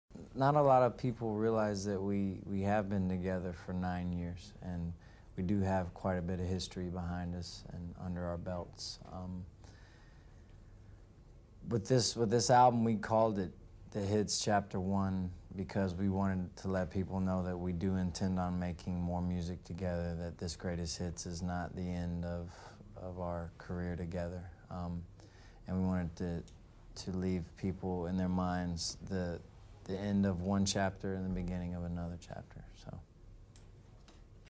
Shining Armor (Voiced by: Kevin Richardson) talks about new Backstreet Boys album
Re-uploaded because of a next scene is glitchy.